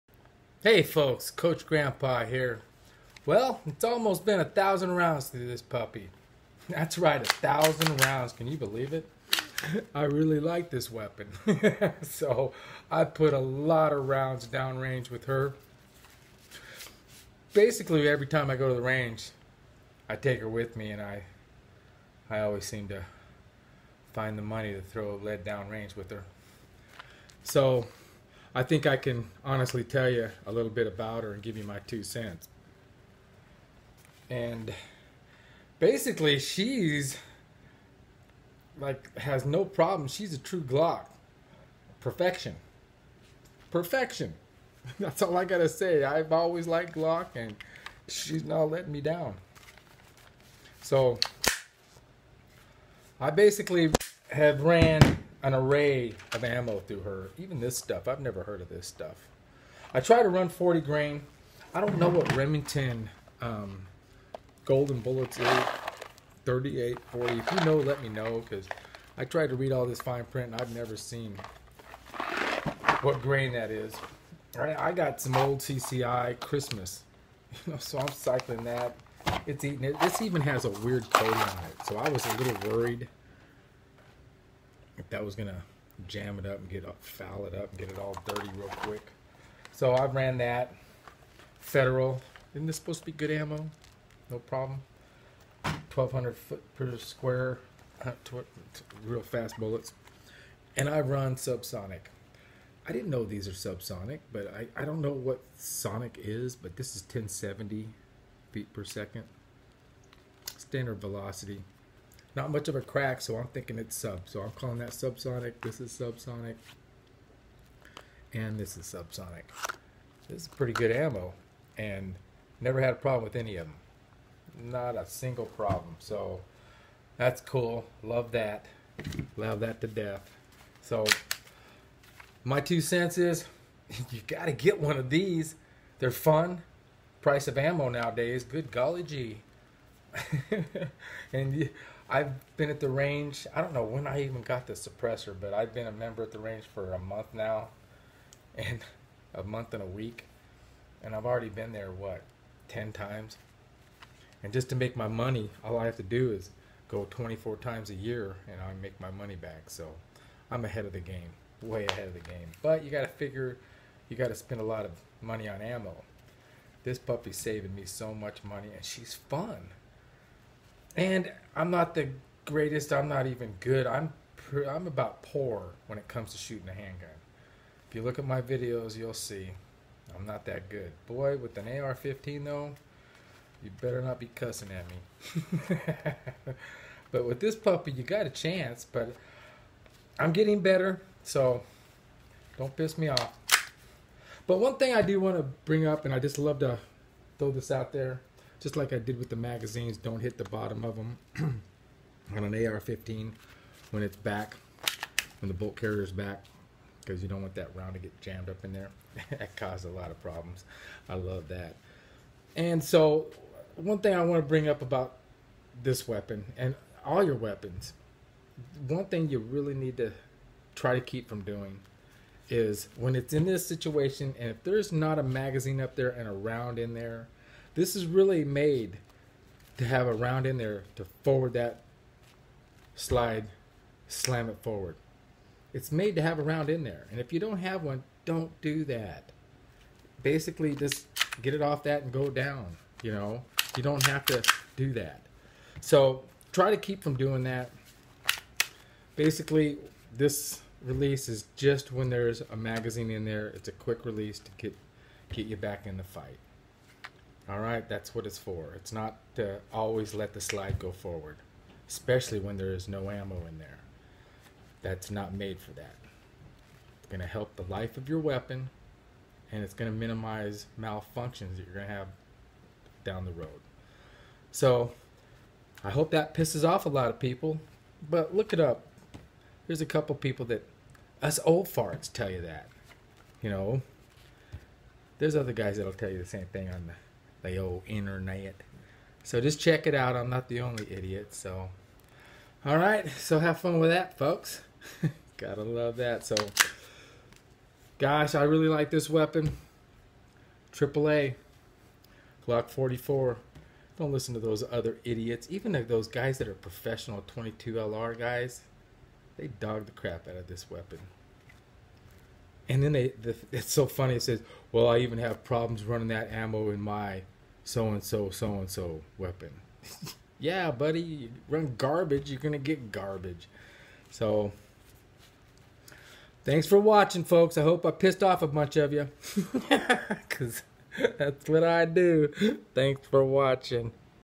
Glock 44 1000 round review,,it is a true Glock
Oh yeah she is...my favorite pistol in the whole world,,,especially with a suppressor from SilencerCo like their sparrow 22..talk about a stapler gun..this puppy is so quiet it's scary..James Bond quiet...LMMFAO  If you don't believe me, go to  Wilson Combat and seach DON"T DO THIS to Your Guns - Gun Guys Ep.  17